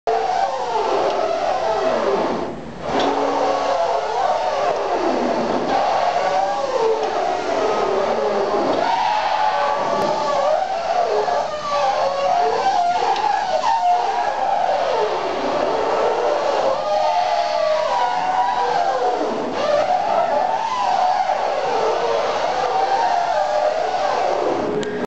Squeaky Ferry
The sound the walking platform makes with the entrance on a windy day.